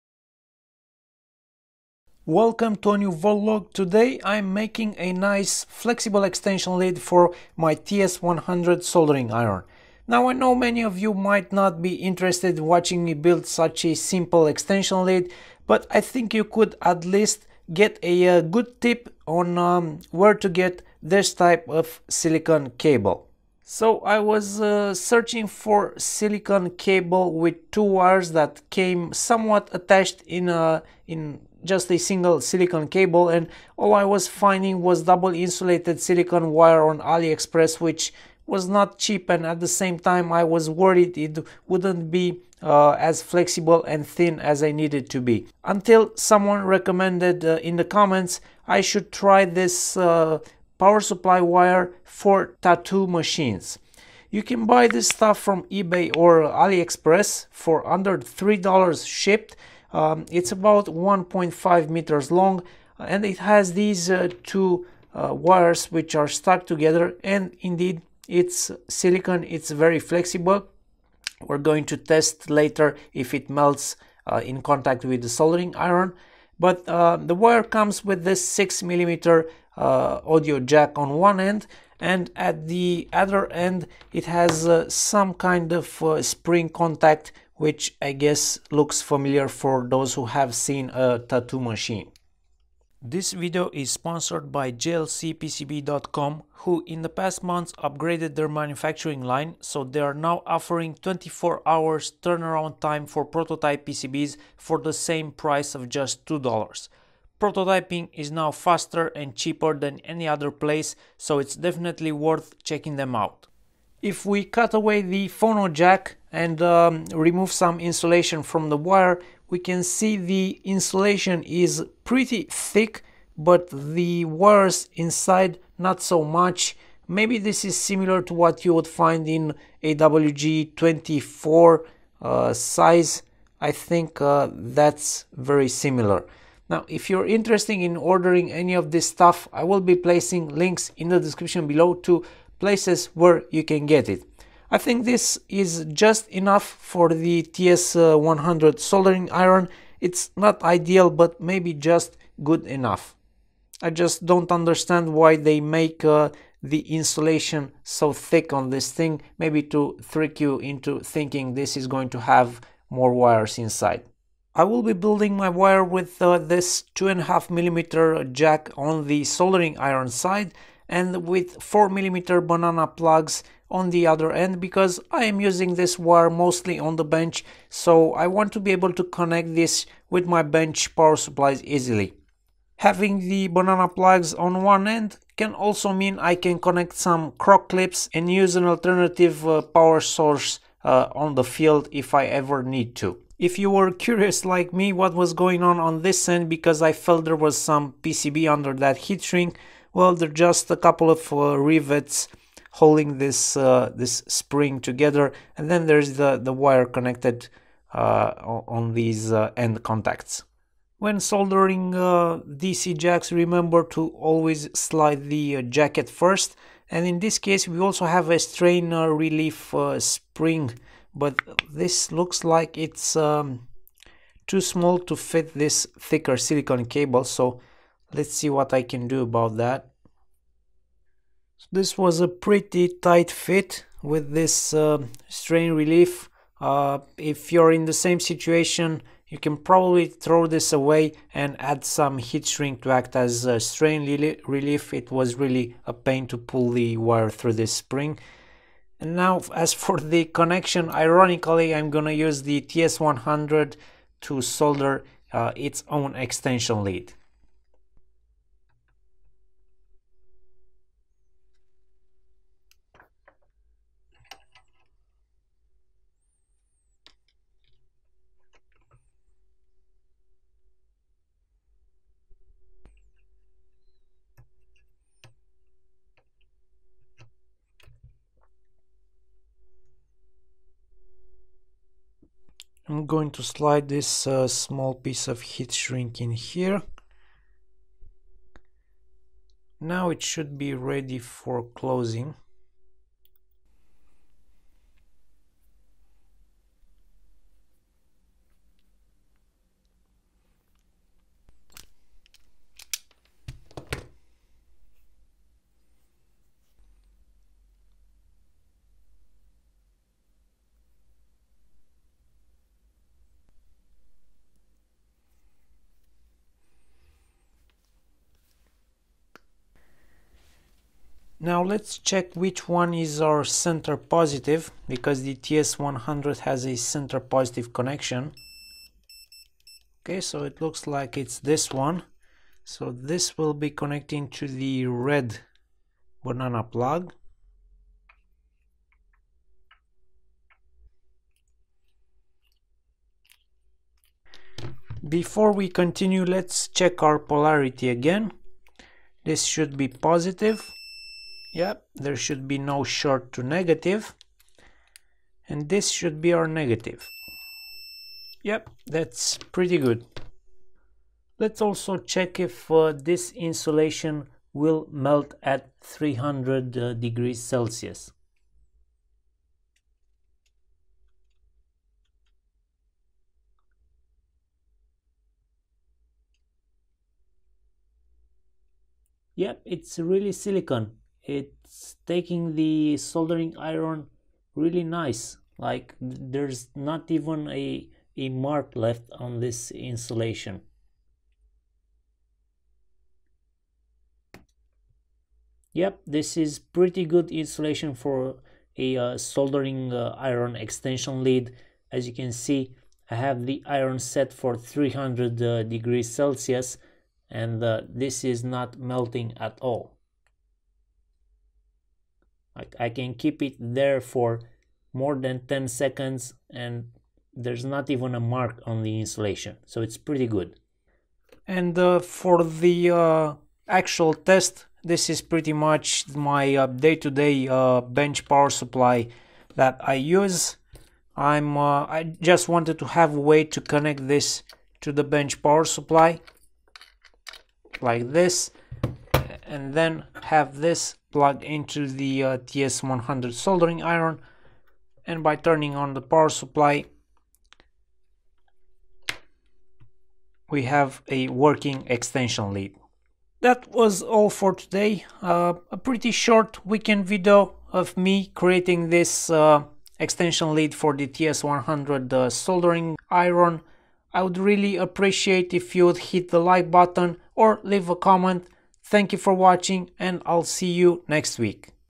Voltlog #212 - DIY Silicone Extension Lead For The TS100 Soldering Iron
$2 for 10 PCBs & $6 for a SMT Stencil Welcome to a new Voltlog, today I’m making a nice flexible extension lead for my TS100 soldering iron